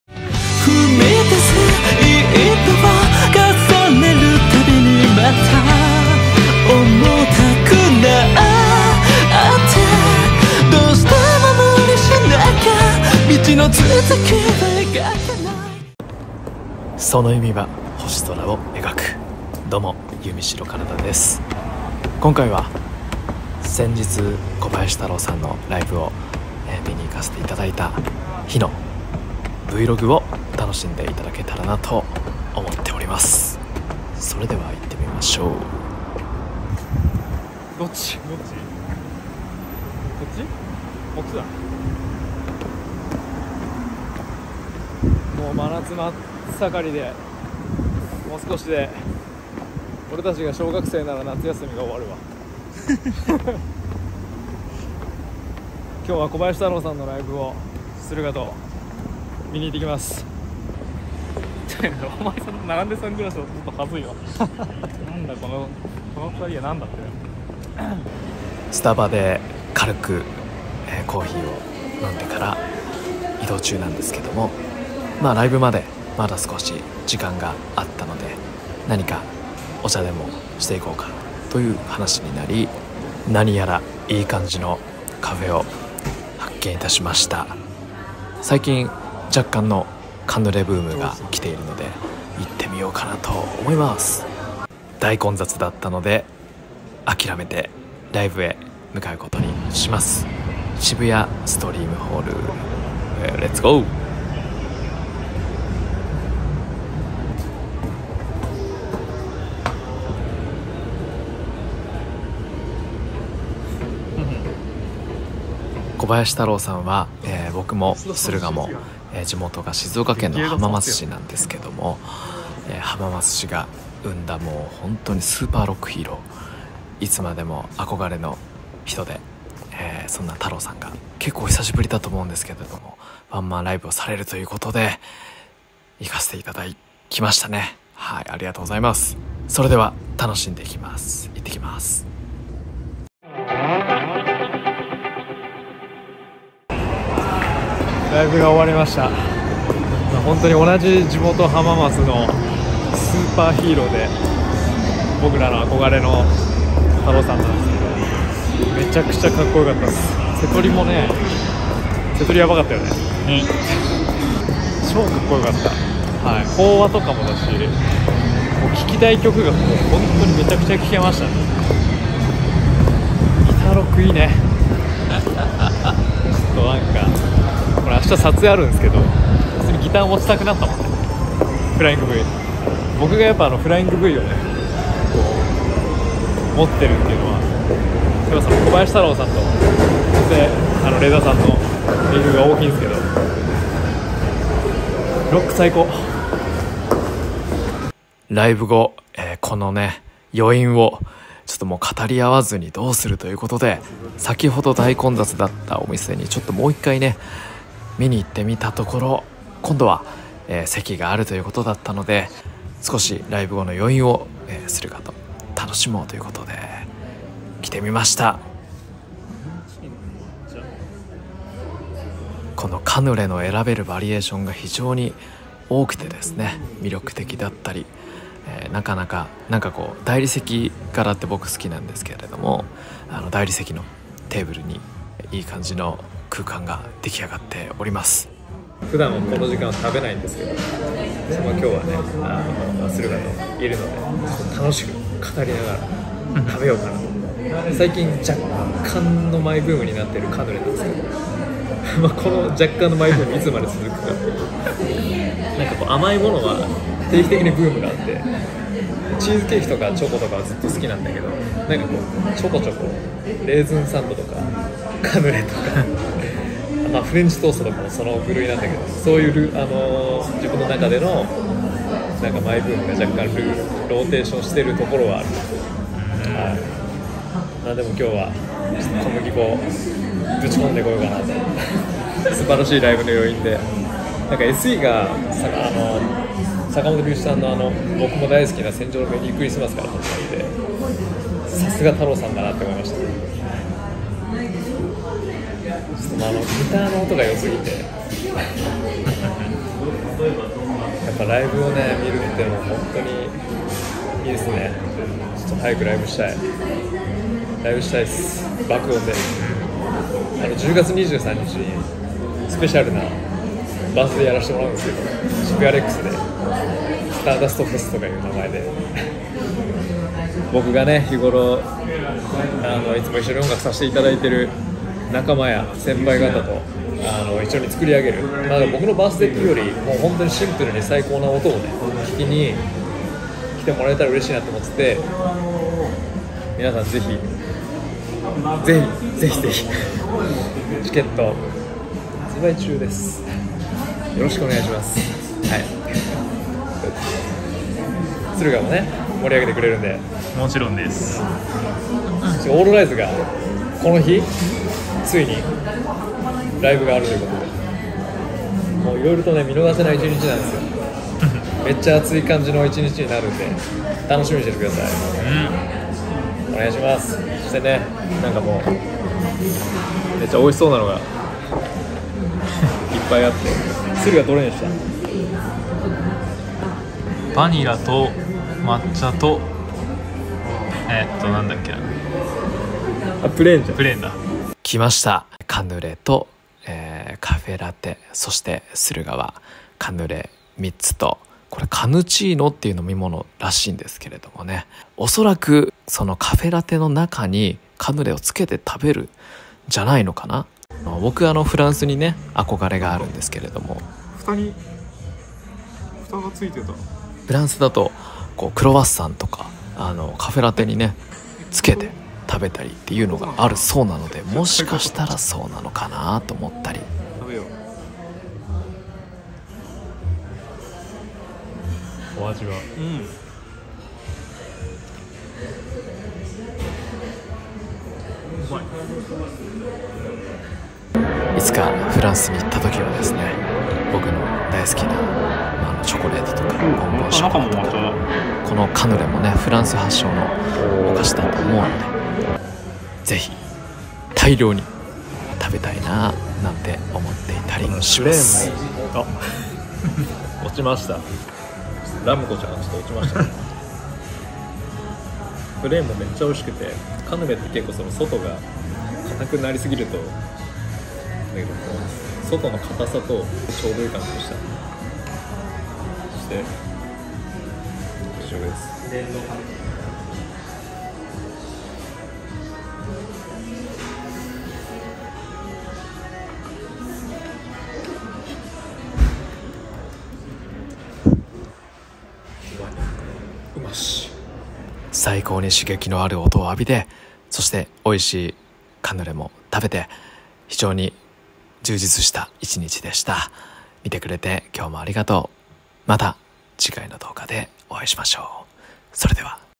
【Vlog】小林太郎さんのライブを観て、カヌレを食べた日
■弓代星空バースデーフェス『STARDUST FES』チケット発売中！

■SURUGAと行ったカヌレカフェ「Canelé de CHIANTI (カヌレ ド キャンティ)」